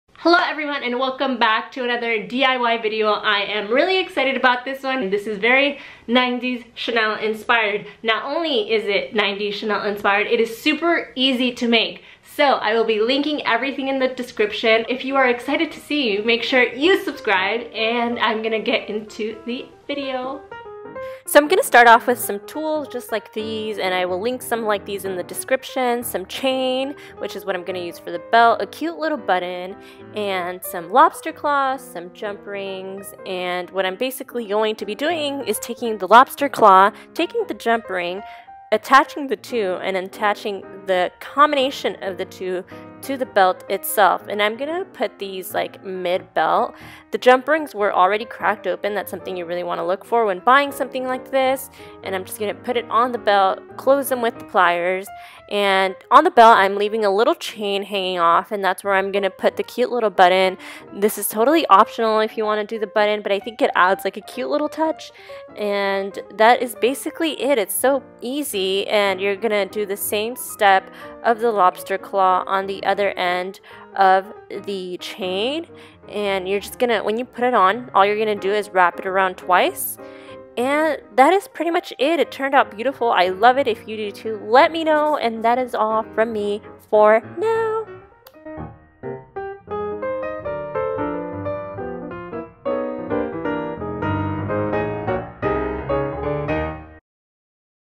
Designer Belt DIY Project!- Designer Inspired Chain Belt
HI everyone!!
I hope you enjoy this designer inspired chain belt! It is such a cute and easy designer belt that can be DIYd at home and worn to any event to make your outfit 10X better.
xoxo. Kat